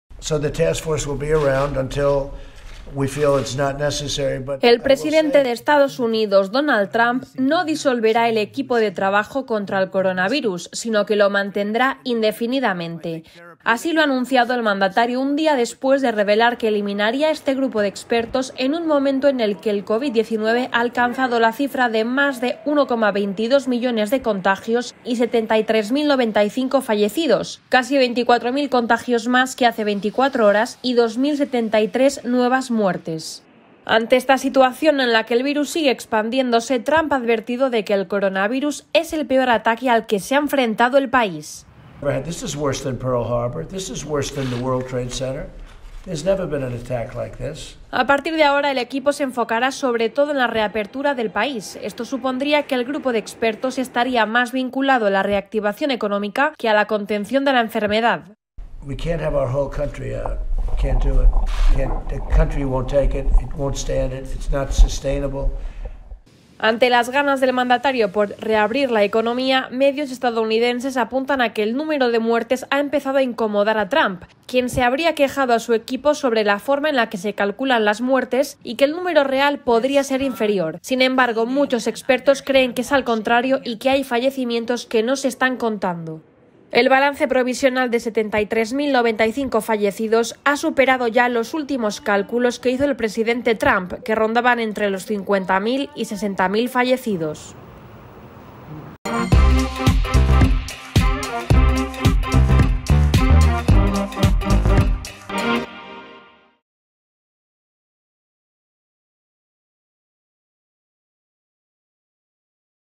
Trump mantiene su sala de crisis contra el coronavirus
El presidente de EE.UU., Donald Trump, dio marcha atrás este miércoles a su plan para desmantelar el equipo de trabajo contra el coronavirus y mantendrá "indefinidamente" esa sala de crisis, mientras admitió que la pandemia ha sido peor para su país que los ataques del 11-S o el de Pearl Harbor. (EFE)